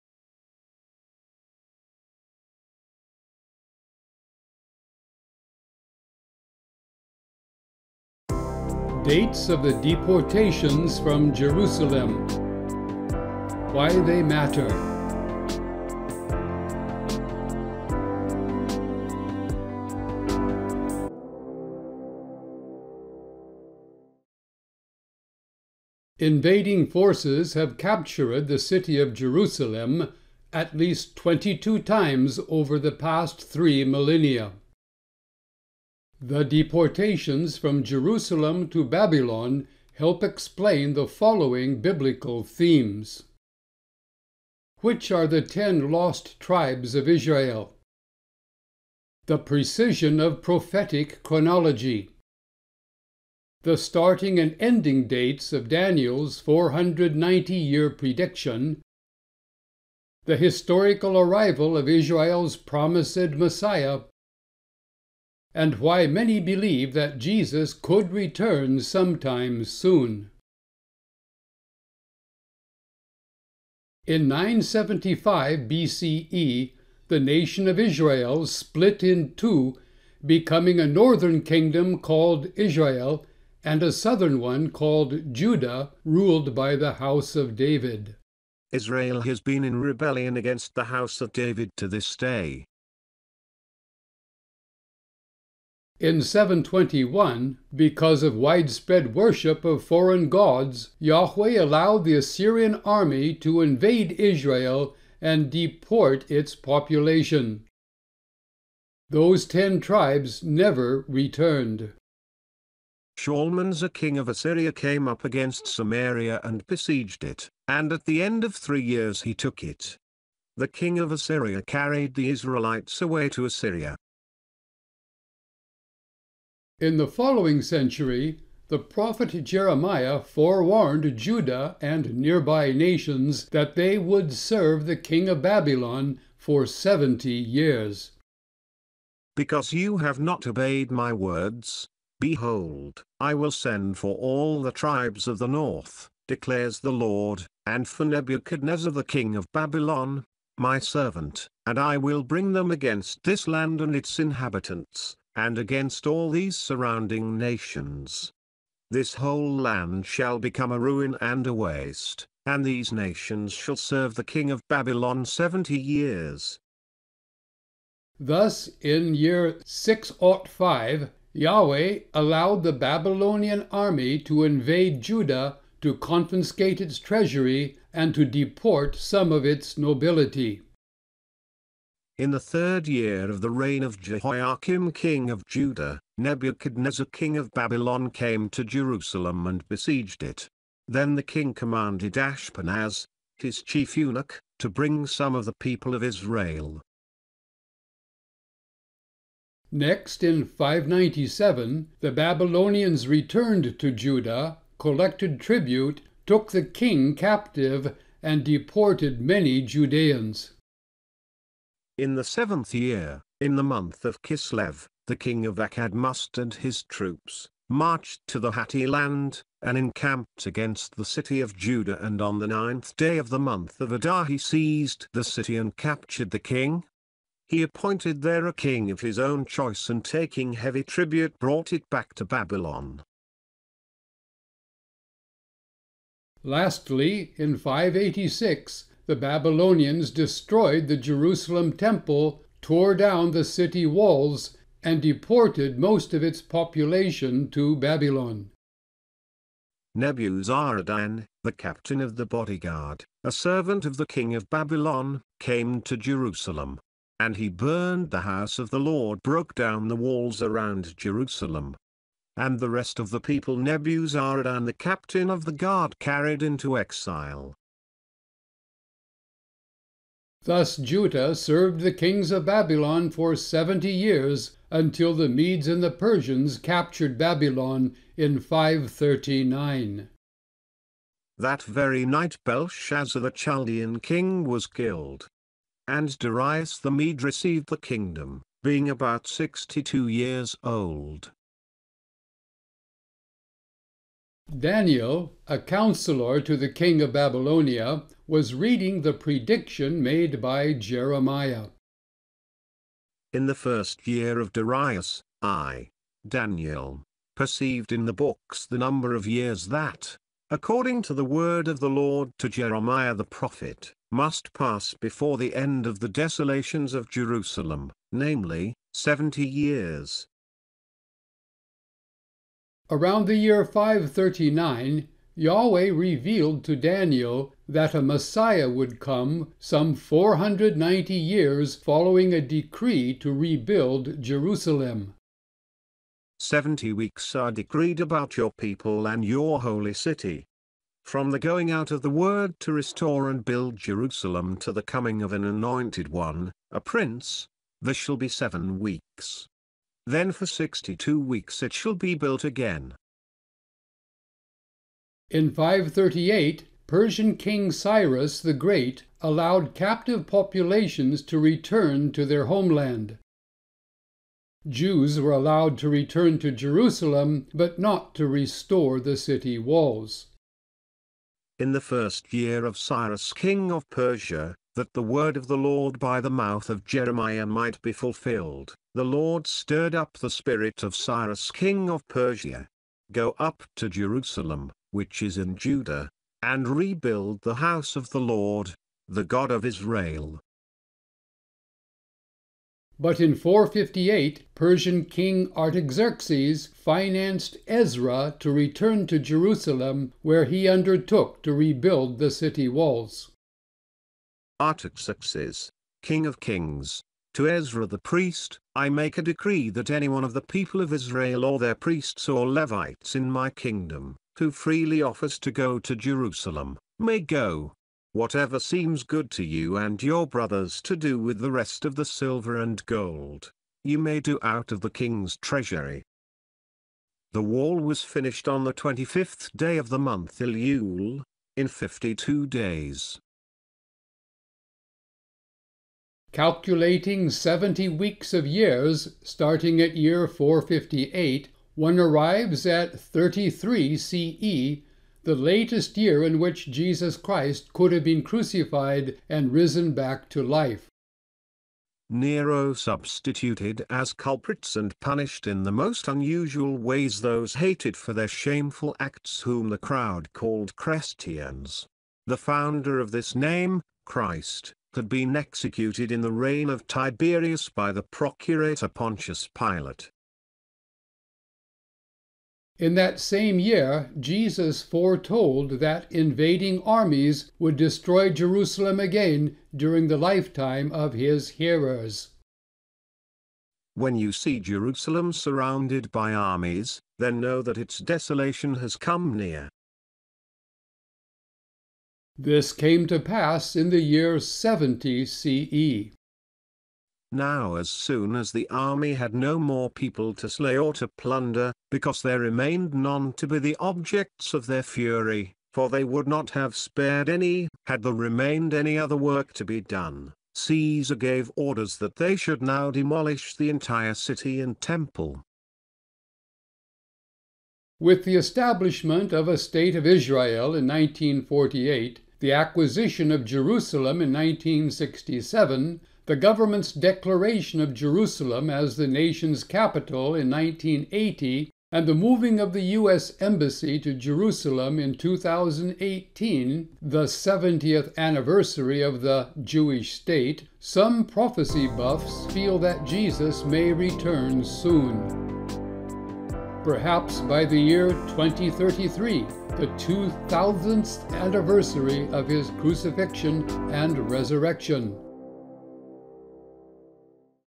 Dates of the Deportations from Jerusalem: Why they matter
Dates of the Deportations from Jerusalem
Why they matter
Bible quotations from the English Standard Version (2016)

Invading forces have captured the city Jerusalem, at least 22 times over the past three millennia.

The deportations from Jerusalem to Babylon help explain the following biblical themes:

● Which are the ten lost tribes of Israel
● The precision of prophetic chronology
● The starting and ending dates of Daniel’s 490-year prediction
● The historical arrival of Israel’s promised Messiah
● Why many believe that Jesus could return sometime soon

In 975 BCE, the nation of Israel split in two, becoming a northern kingdom called Israel, and a southern one called Judah ruled by the House of David. 2 Chronicles 10:19

In 721, because of widespread worship of foreign gods, Yahweh allowed the Assyrian army to invade Israel and deport its population. These ten tribes never returned. 2 Kings 18:9, 11

In the following century, the prophet Jeremiah forewarned Judah and nearby nations that they would serve the king of Babylon for seventy years. Jeremiah 25: 8-9, 11

Thus, In 605, Yahweh allowed the Babylonian army in invade Judah, to confiscate its treasury, and to deport some of its nobility. Daniel 1:1-3

Next, in 597, the Babylonians returned to Judah, collected tribute, took the king captive, and deported many Judeans. Babylonian Chronicles, 293; 2 Kings 24:12; Jeremiah 52:28, 30

Lastly, in 586, the Babylonians destroyed the Jerusalem temple, tore down the city walls, and deported most of its population to Babylon. 2 Kings 25:8-11

Judah served the kings of Babylon for seventy years, until the Medes and the Persians captured Babylon in 539. Daniel 5:30-31

Daniel, a counsellor to the King of Babylonia, was reading the prediction made by Jeremiah. Daniel 9:1-2

Around 539, Yahweh revealed to Daniel that a messiah would come 490 years following a decree to rebuild Jerusalem. Daniel 9:24-25

In 538, Persian king Cyrus the great allowed captive populations to return to their homeland. Jews were allowed to return to Jerusalem, but not to restore the city walls. Ezra 1:1-3

But in 458, Persian king Artaxerxes financed Ezra to return to Jerusalem, where he undertook to rebuild the city walls. Ezra 7:12-20; Nehemiah 6:15

Calculating 70 weeks of years, starting at year 458, one arrives at 33 CE, the latest year in which Jesus Christ could have been crucified and risen back to life. Gaius Cornelius Tacitus, Annals XV.44

In that same year, Jesus foretold that invading armies would destroy Jerusalem again during the lifetime of his hearers. Luke 21:20

This came to pass in the year 70 CE. BJ Josephus, Jewish Wars, 7:1:1

With the establishment of a state of Israel in 1948, the acquisition of Jerusalem in 1967, the government’s declaration of Jerusalem as the national capital in 1980, and the moving of the U.S. embassy to Jerusalem in 2018, the 70th anniversary of the “Jewish state,” some prophecy buffs feel that Jesus may return soon.

Perhaps by the year 2033, the 2000th anniversary of Jesus’ crucifixion and resurrection?